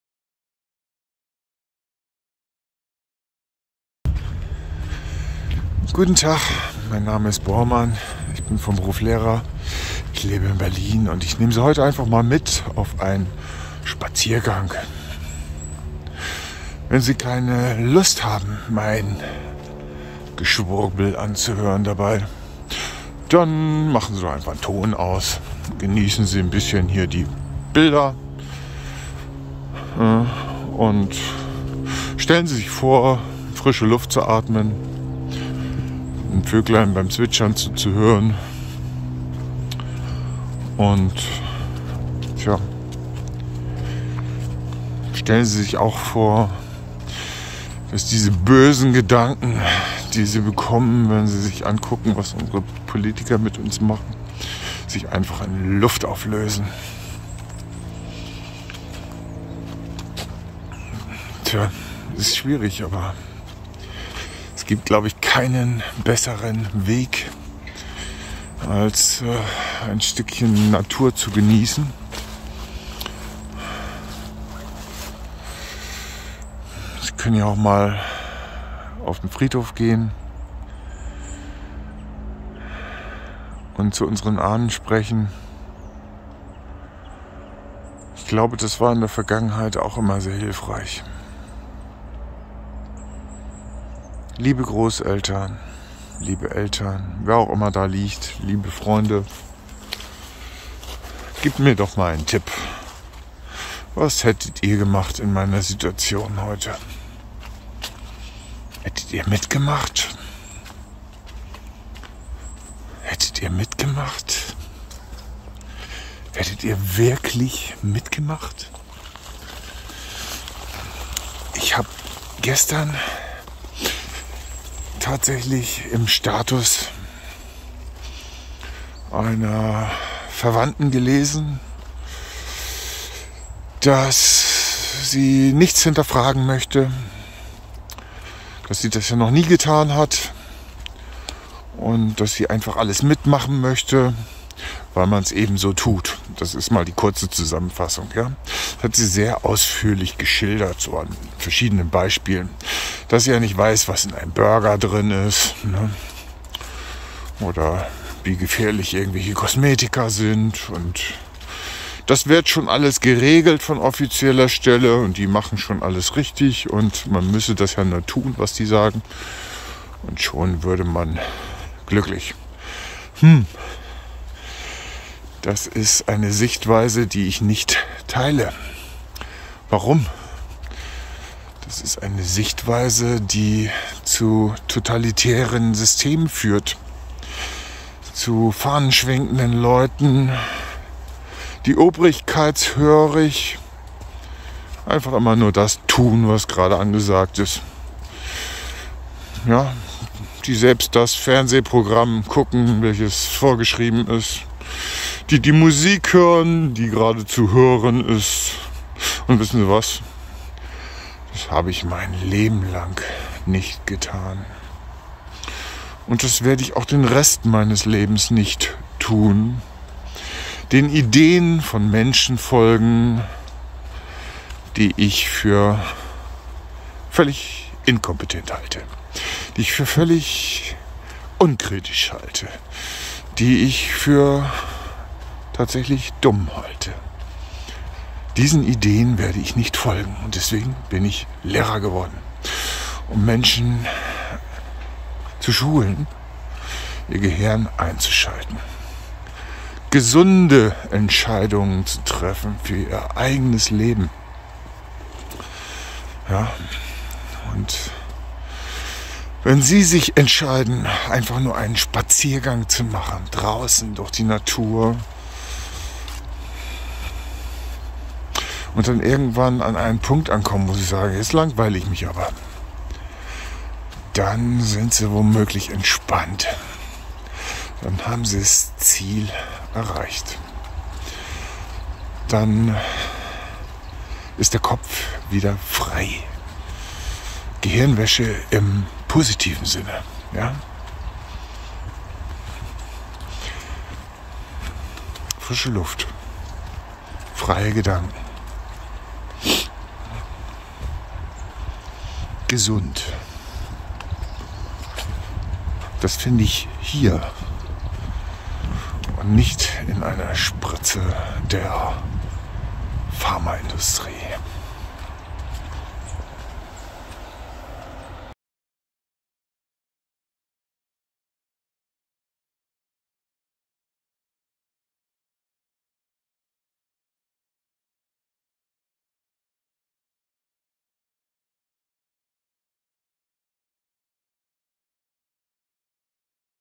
Sauerstoff tanken, Synapsen aktivieren!
Der Begriff "Gehirnwäsche" ist im Allgemeinen negativ besetzt: "... den psychischen Widerstand mit gewaltsamer Einwirkung zu brechen. Theorien der Gehirnwäsche entstanden zunächst im Zusammenhang mit totalitären Staaten ..."
Sind wir aktuell Opfer einer massiven täglichen "Gehirnwäsche" nach klassischer Definition? Blinder Kadavergehorsam ist das Gegenteil eines gesunden Verstandes. Ich verwende den Begriff "Gehirnwäsche" heute einmal positiv. Unser Hirn braucht Sauerstoff! Ein freies und mit Sauerstoff "gewaschenes" und versorgtes Gehirn generiert kreative Gedanken! Das ist es, was die Welt jetzt braucht. Ich nehme Sie mit auf einen Spaziergang an der frischen Luft ...